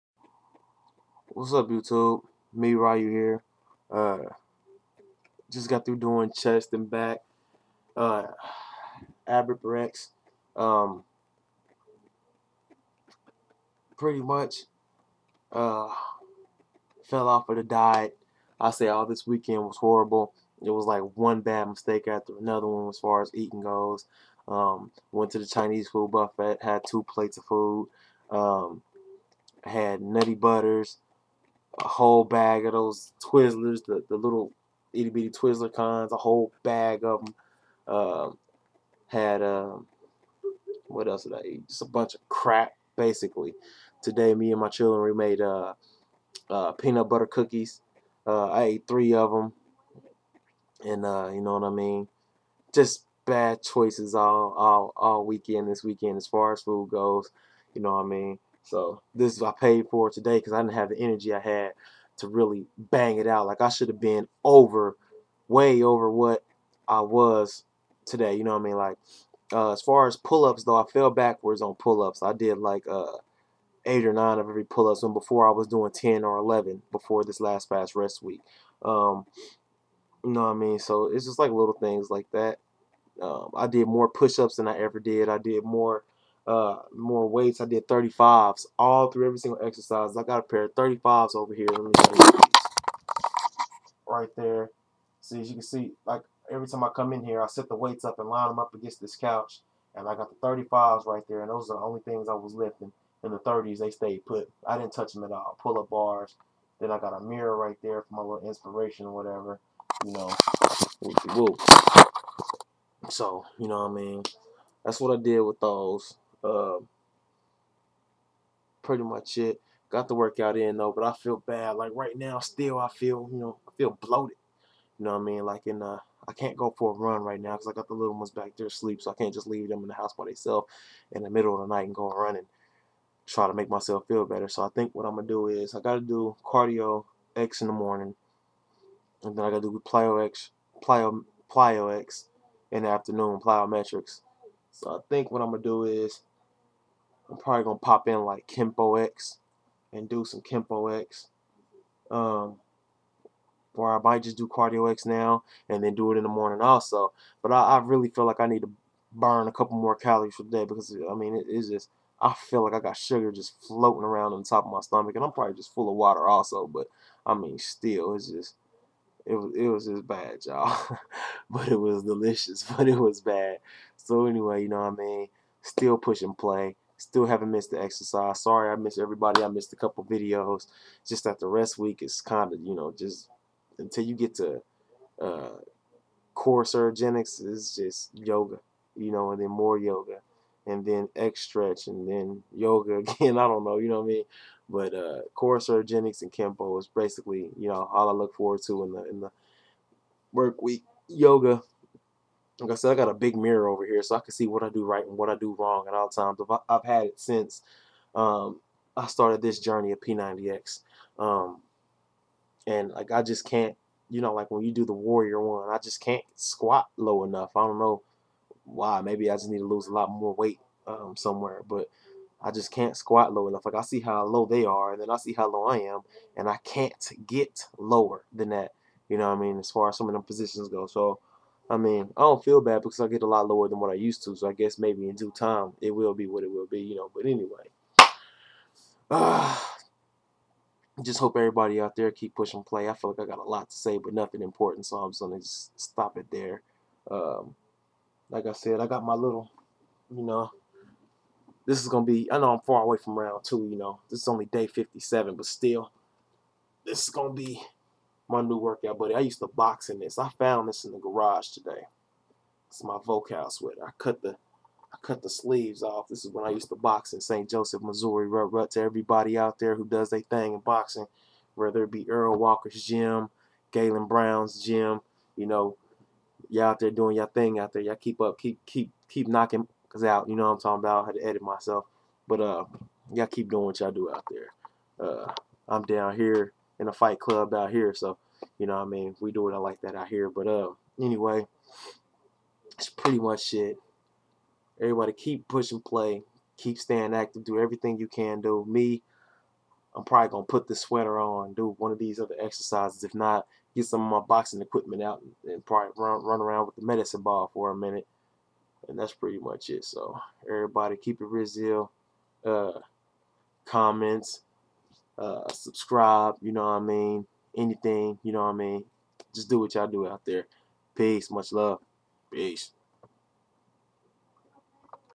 P90X Day 57
Hi my name is Ryu and I've been clean for 81 days. No sugars no simple carbs no sodas just clean. But this weekend I... I... I fell off the wagon. I ate sweets all week end long I'm so ashamed of myself I was so week but the sugar keep calling me every were I went it was there and it knew my name.